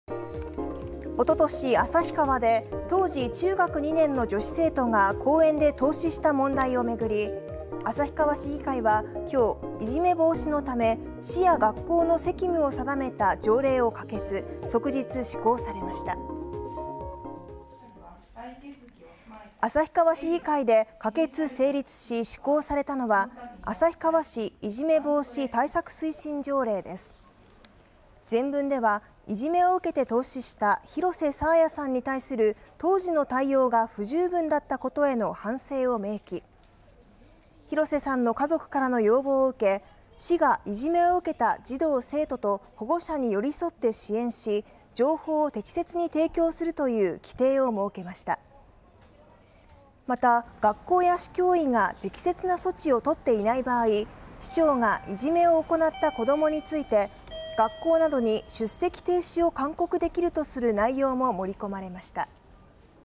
旭川市議会が「いじめ防止条例案」可決 中２女子凍死問題うけ、当時の不十分な対応への反省明記
おととし旭川で、当時中学２年の女子生徒が公園で凍死した問題をめぐり、旭川市議会は30日、いじめ防止のため市や学校の責務を定めた条例を可決、即日施行されました。2023年06月30日(金) 19時04分 更新